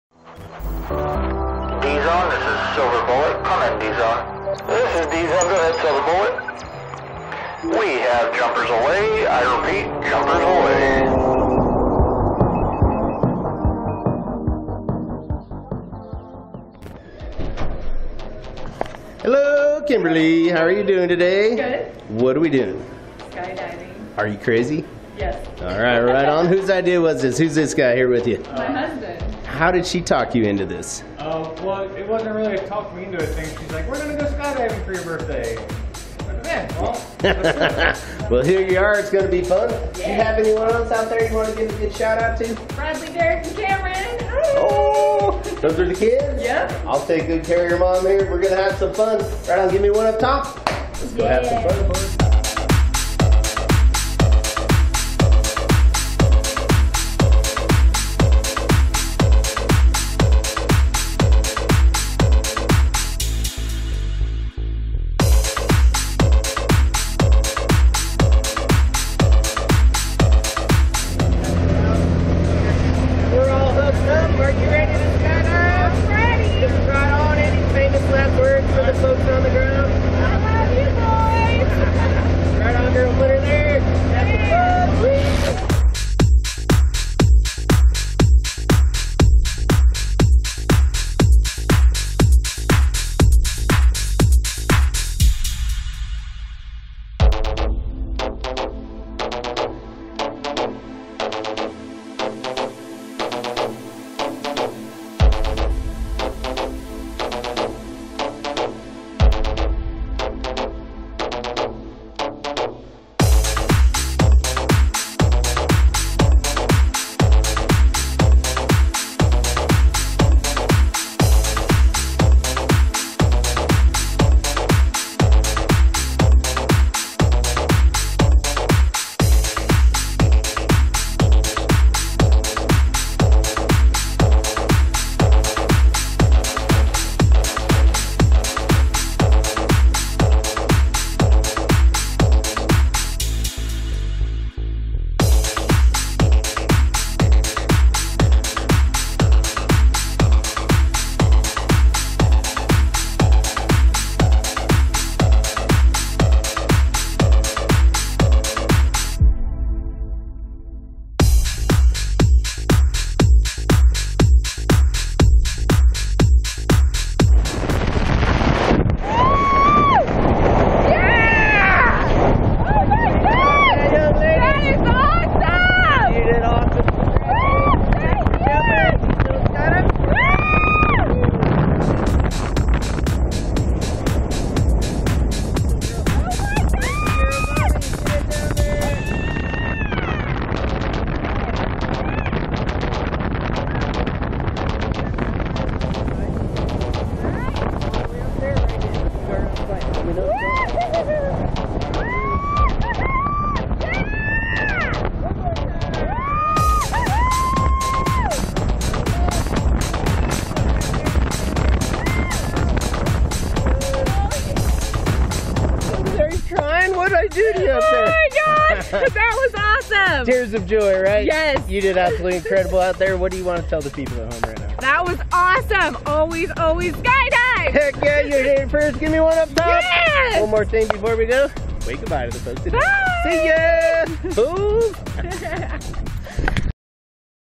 Kimberly Duffy's Tandem skydive!
Kimberly Duffy's tandem skydive on Friday, July 17, 2020 with Jeremy Divan.